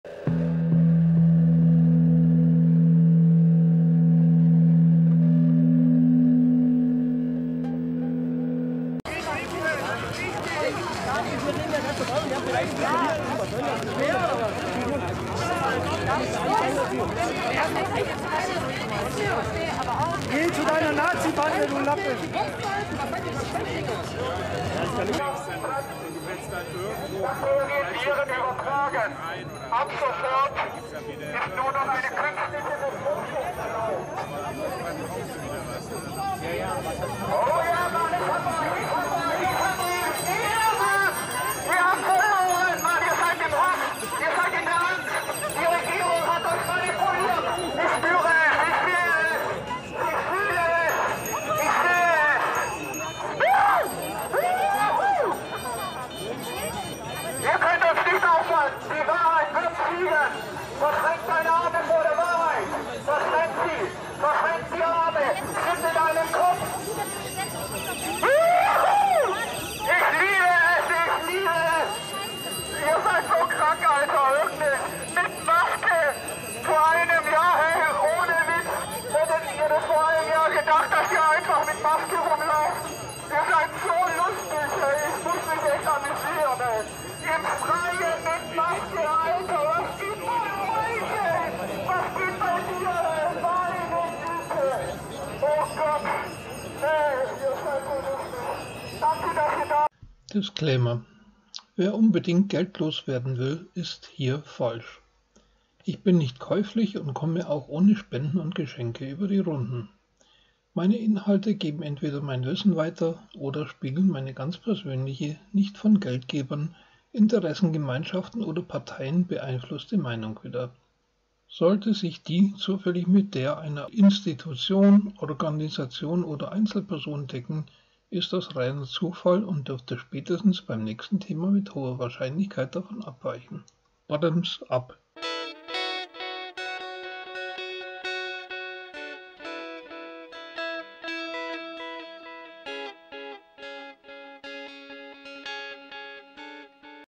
Durchgeknallter Covidiot in Aktion
Dieses Hüpfemännchen ist so bescheuert, dass es sich ein eigenes Video verdient hat. Mehr Worte sind nicht notwendig...